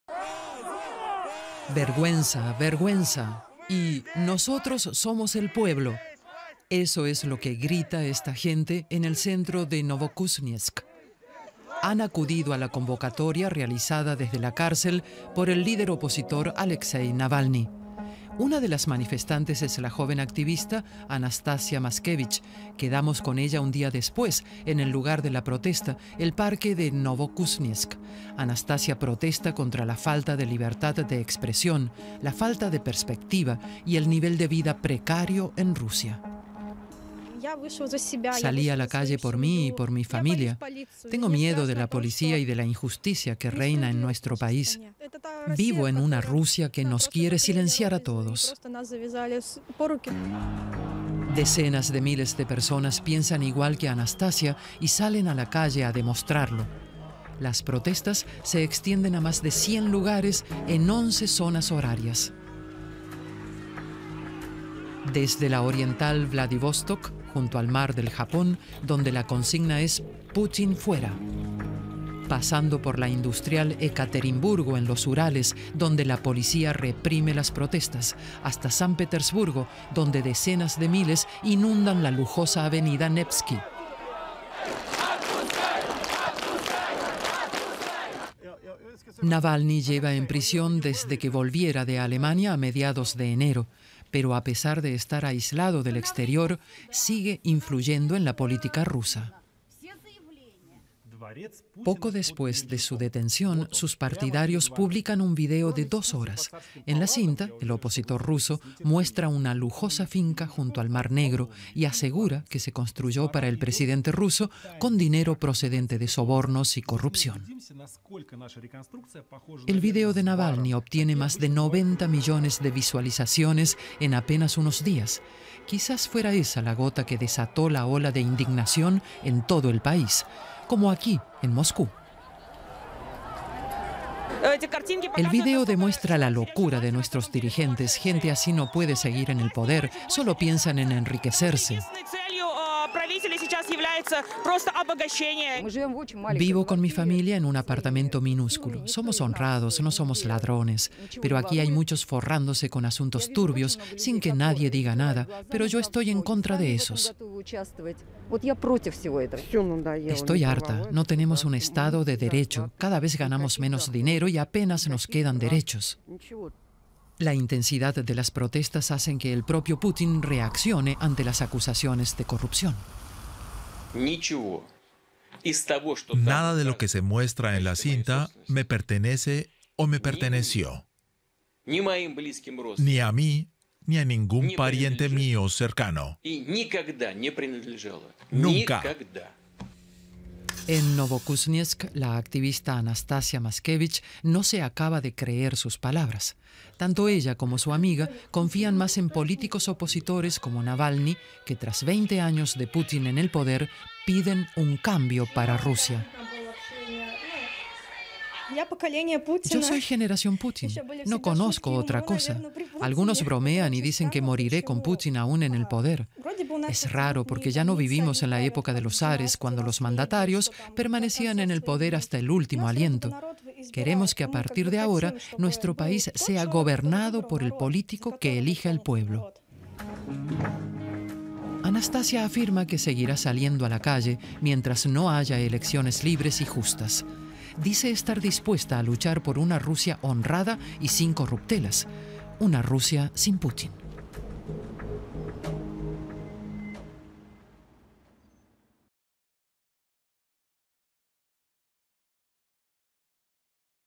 Rusia: protestas contra Putin
El opositor ruso, Alexei Navalni logra movilizar desde la cárcel a sus seguidores logrando la mayor protesta de los últimos años en el país. Decenas de miles de personas se reunieron en más de 80 ciudades por todo el país.